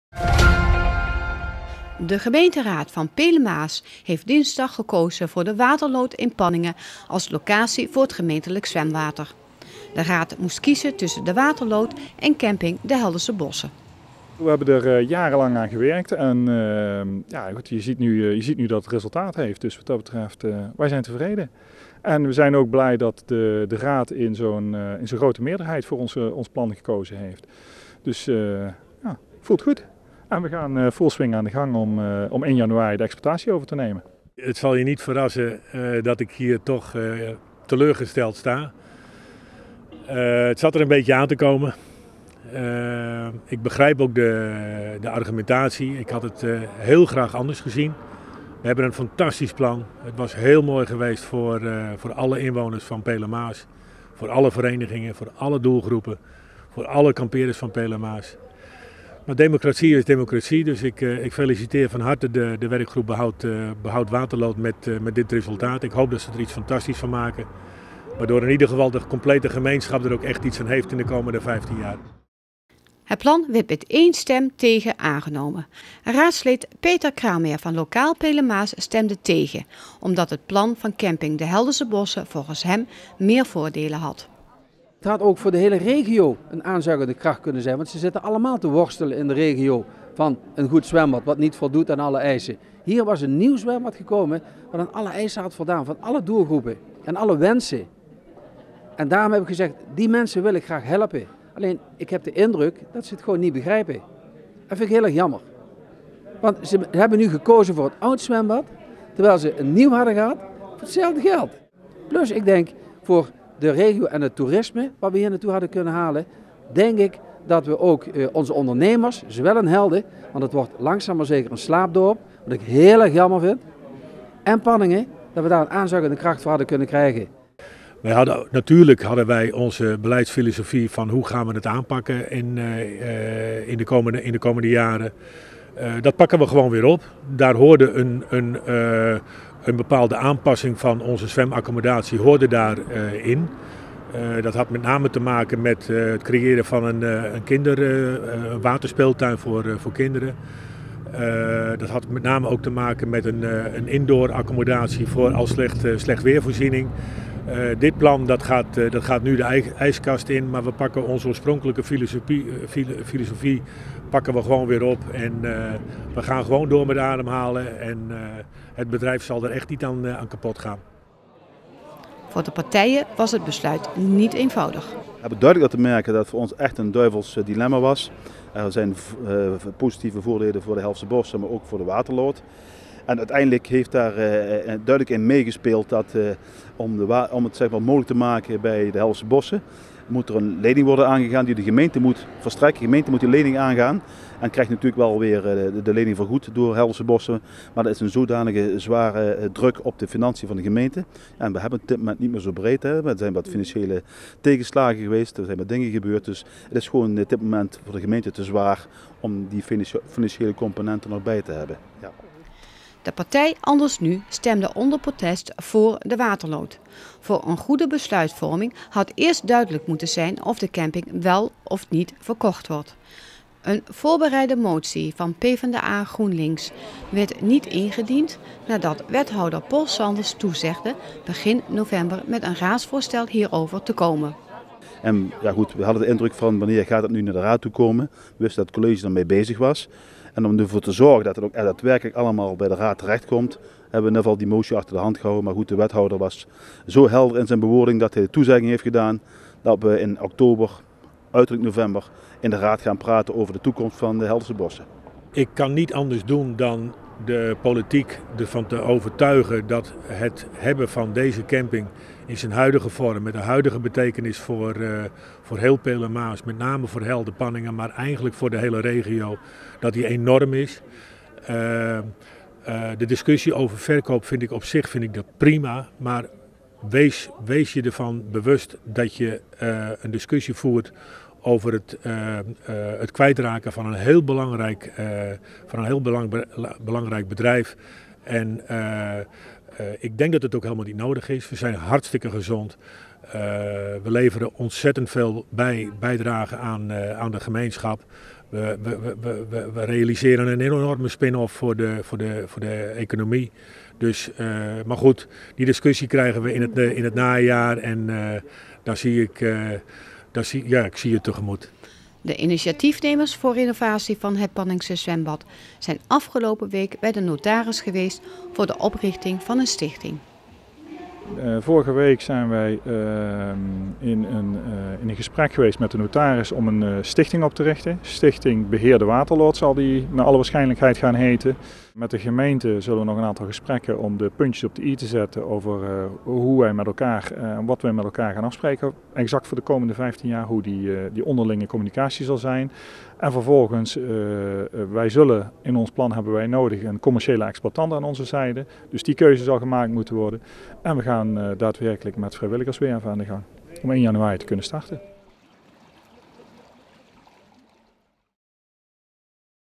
Raad kiest voor Zwembad de Waterloat
De gemeenteraad van Peel en Maas heeft dinsdag gekozen voor de Waterloat in Panningen als locatie voor het gemeentelijk zwemwater. Het plan werd met één stem tegen aangenomen. De raad moest kiezen tussen De Waterloat en camping De Heldense Bossen.

De initiatiefnemers voor renovatie van het Panningse zwembad zijn afgelopen week bij de notaris geweest voor de oprichting van een stichting. Stichting Beheer De Waterloat, zoals de stichting waarschijnlijk gaat heten, zal de exploitatie van het zwembad in januari overnemen. Sietse Tienkamp, directeur van de Heldense Bossen reageerde teleurgesteld, maar kon de argumentatie van de raadsleden ook begrijpen.